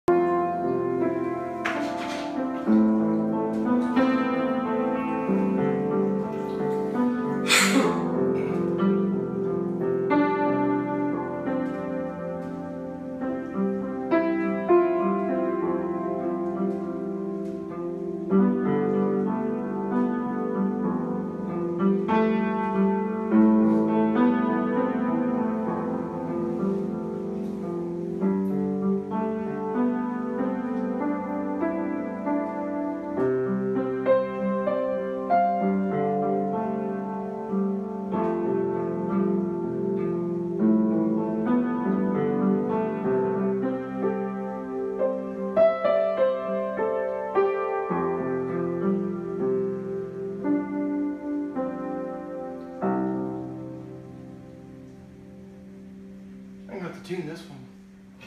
Houston Musician Plays Piano After Returning to Flooded Home
Courtesy of Aric Harding